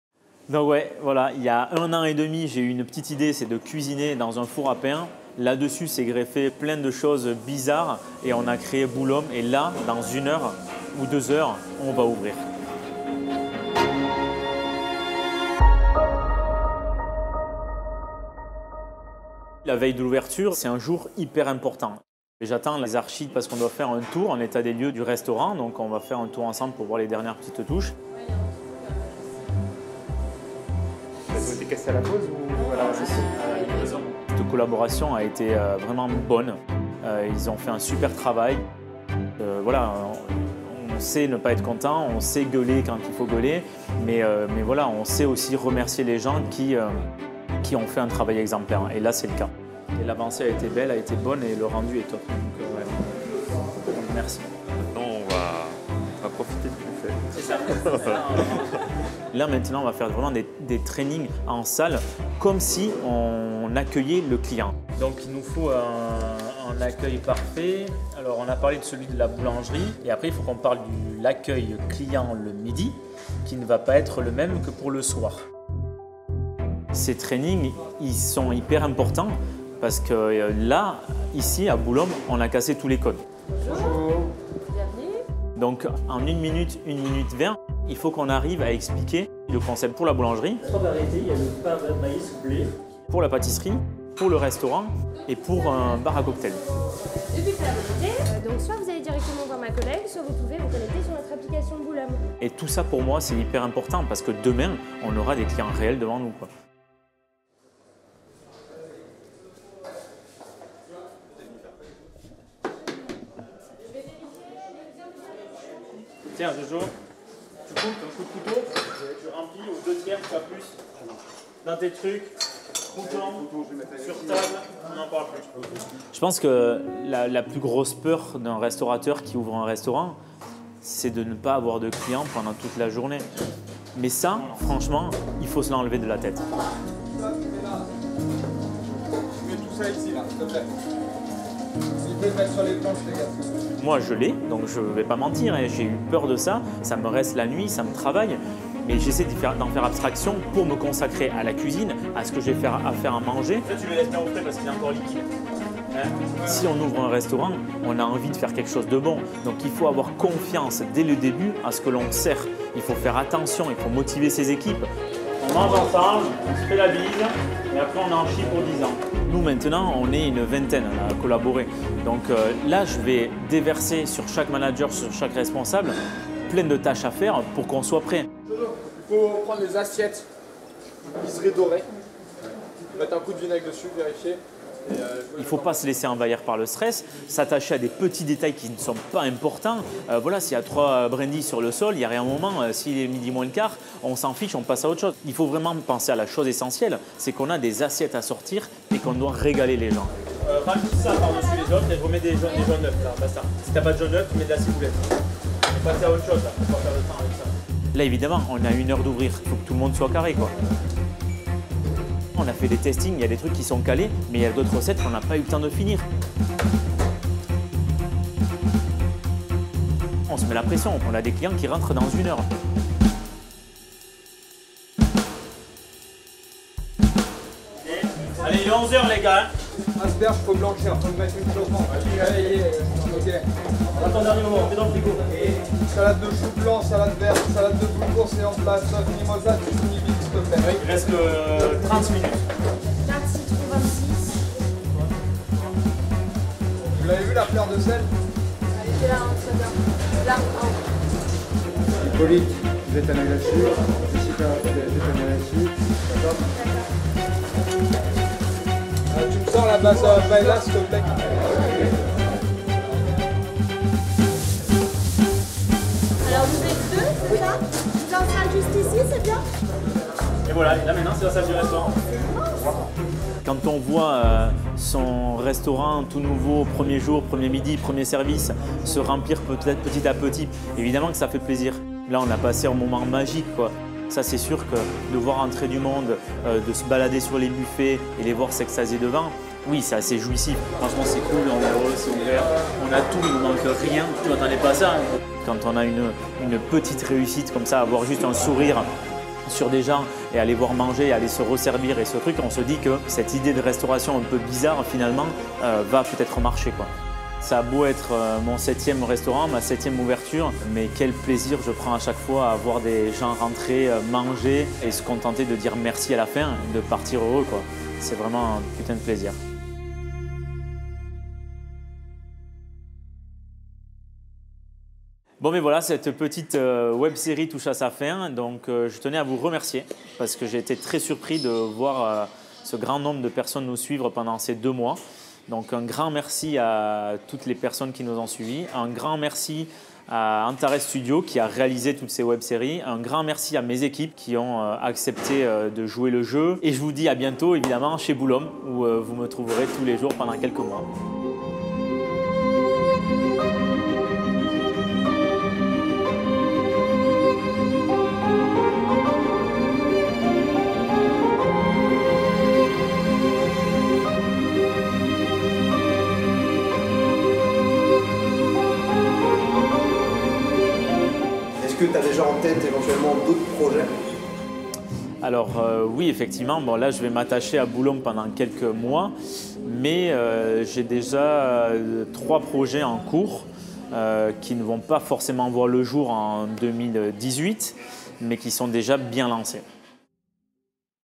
BOULOM Project - Julien Duboué (Episode 9)
Cette semaine, je vous parle des différents plats qui vous attendent maintenant chez Boulom ainsi que de la phase de testing qui a eu lieu avec toute l'équipe.
Vous pourrez aussi découvrir certains vins et cocktails du restaurant !